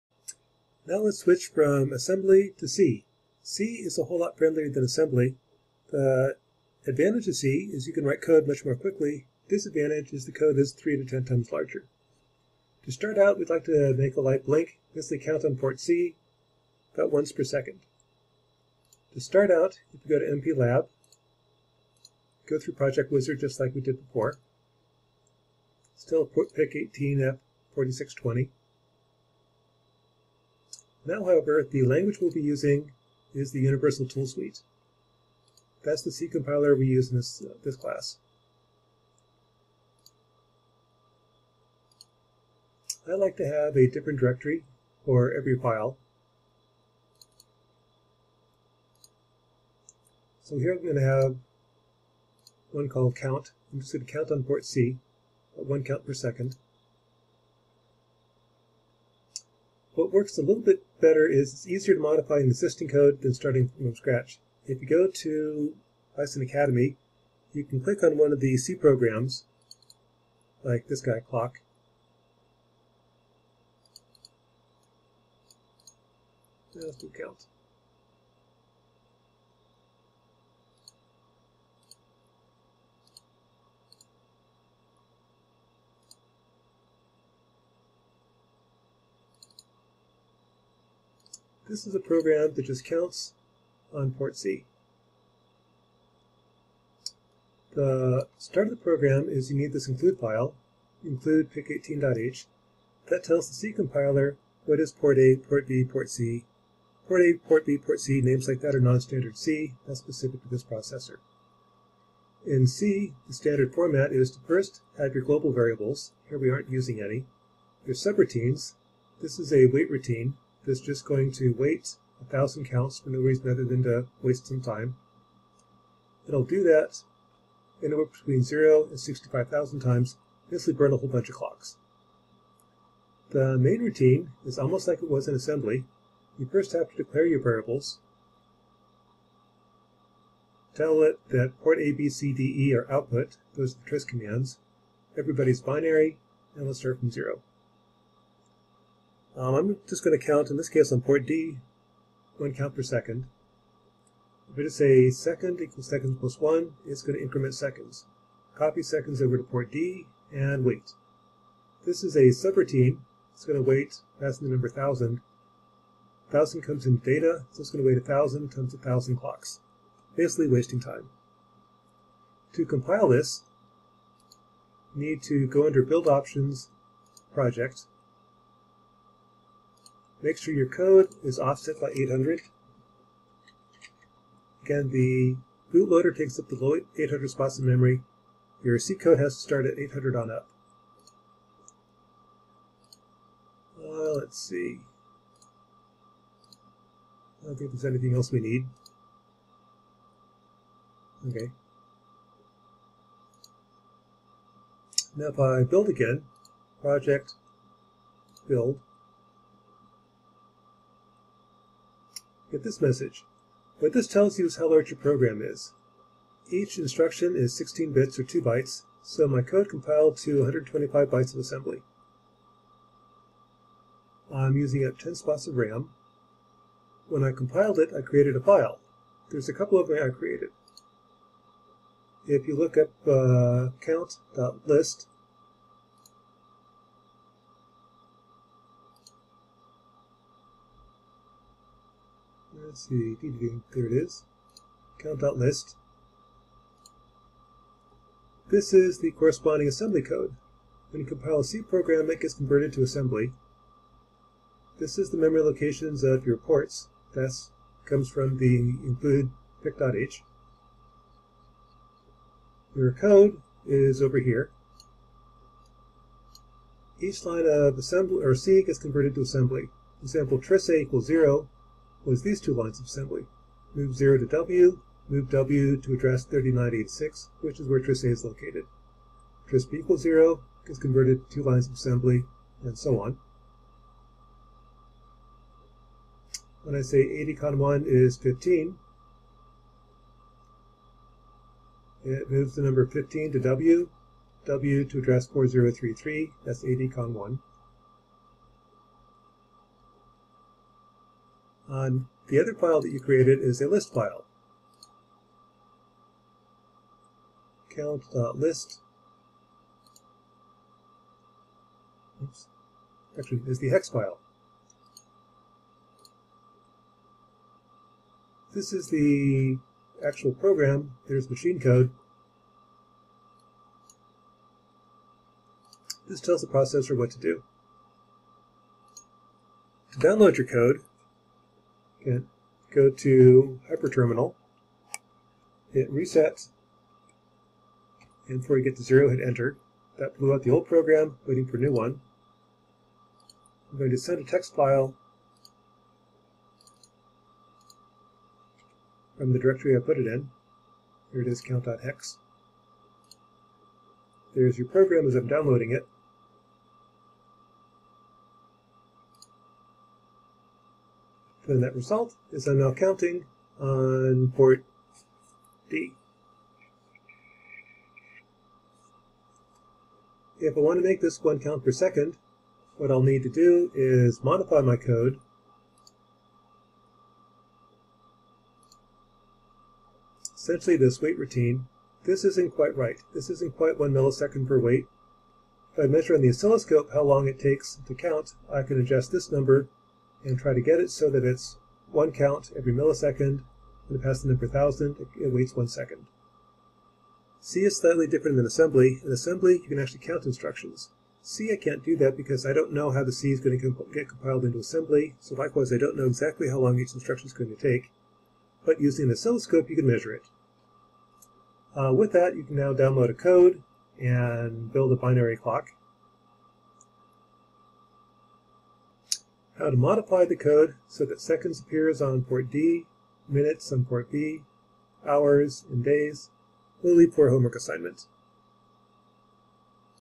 How to Download a C Program to your PIC18F4620 EVB
Demonstration of compiling and downloading a C program to your PIC18F4620 EVB using MPLAB8.92. NDSU / ECE 376 Embedded Systems - lecture notes located